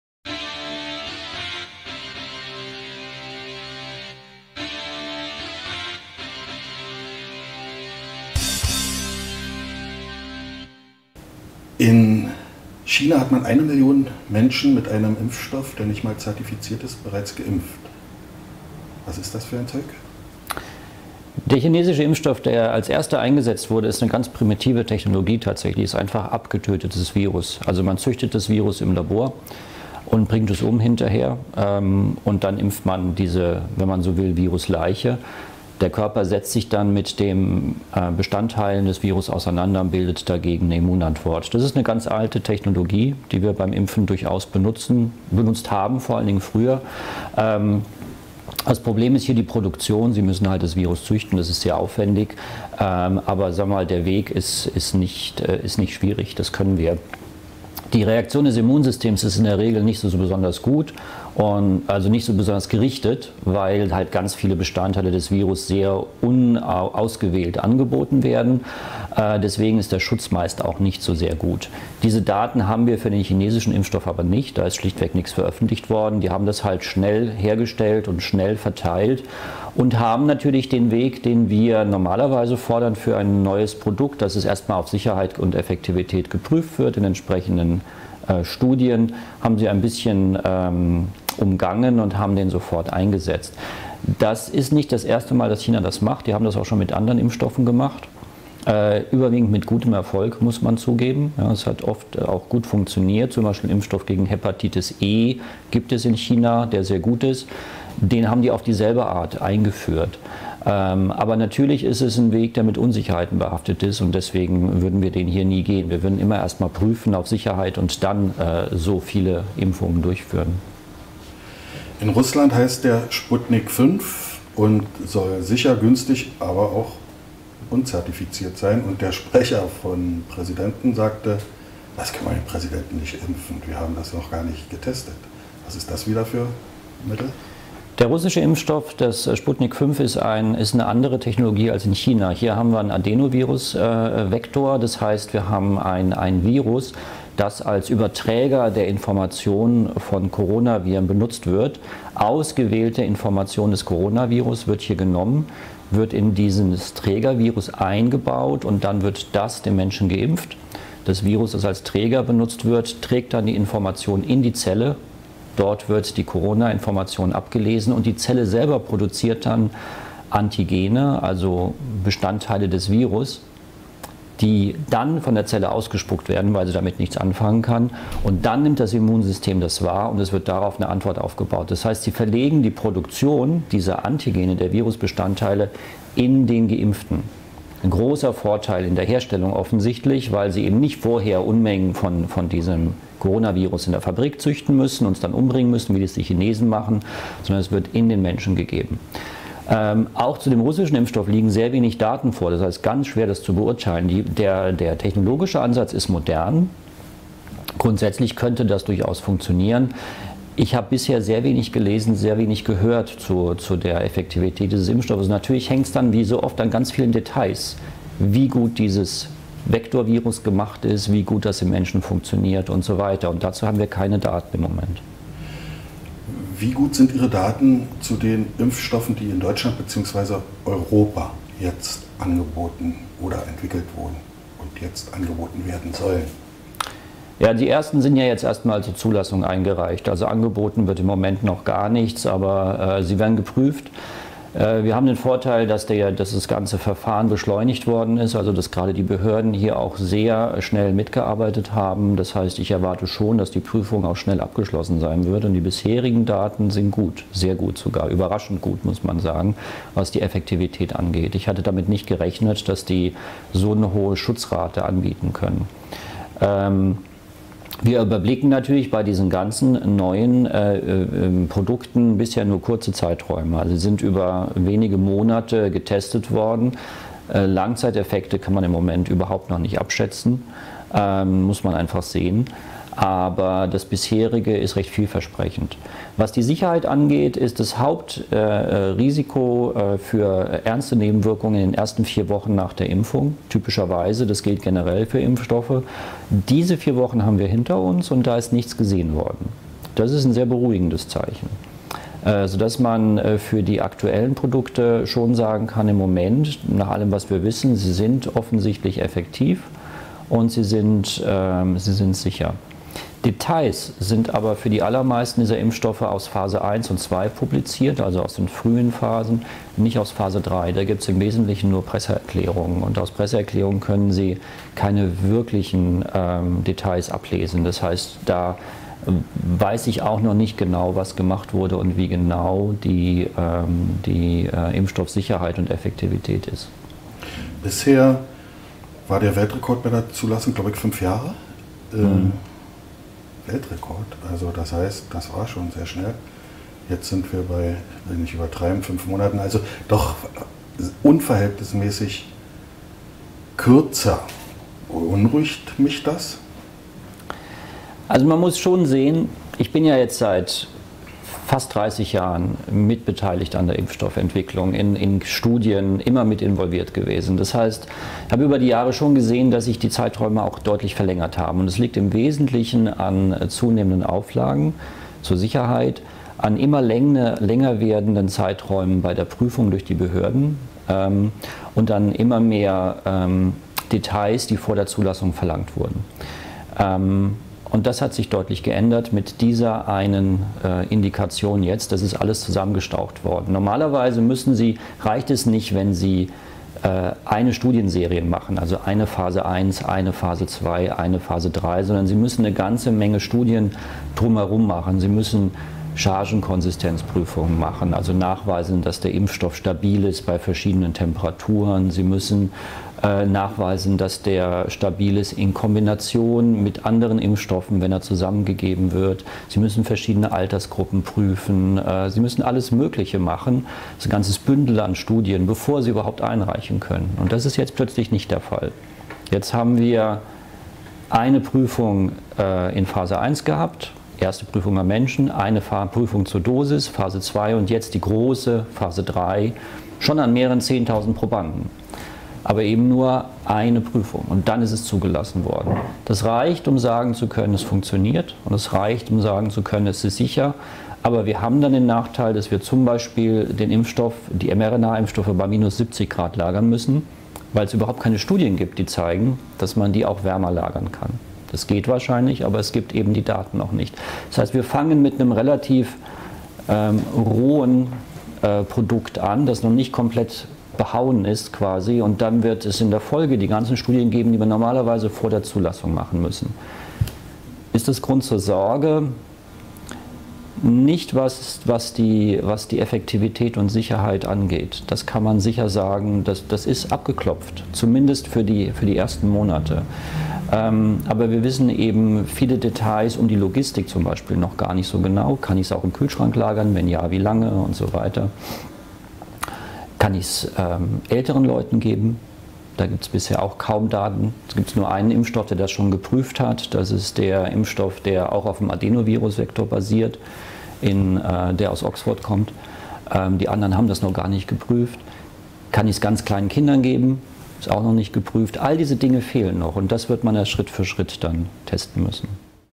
Corona-Impfstoff Spezial Teil 1 - Studienlage zu den bekannten Impfstoffen
In unserem Corona-Impfstoff Spezial dreht sich alles um die neu entwickelten Impfstoffe gegen das Corona-Virus.
Wie gut sind die Impfstoffe aus China & Russland? Wie ist die aktuelle Studienlage zur Wirksamkeit und Sicherheit der europäischen Kandidaten?
Prof. Dr. Jelinek, der selber seit Jahren an der Impfstoffentwicklung beteiligt ist, hat Antworten für uns!